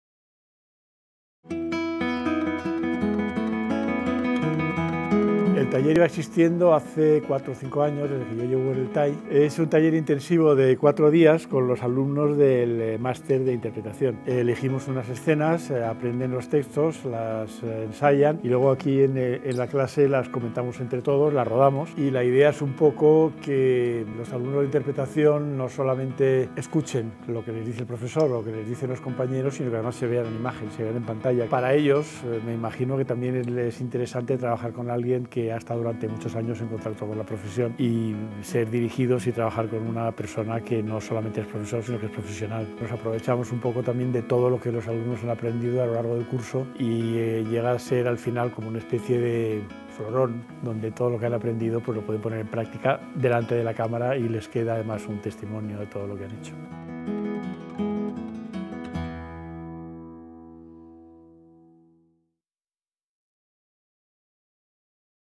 Taller de Interpretación ante la Cámara con JAIME CHÁVARRI
Taller intensivo de 4 días que realizan los alumnos del Master en Master en Actuación Cinematográfica del Área de Artes Escénicas de la Escuela TAI con el director de cine, Jaime Chávarri, sobre cómo enfrentarse ante la cámara a partir de textos de escenas que ruedan para posteriormente análizarse en pantalla.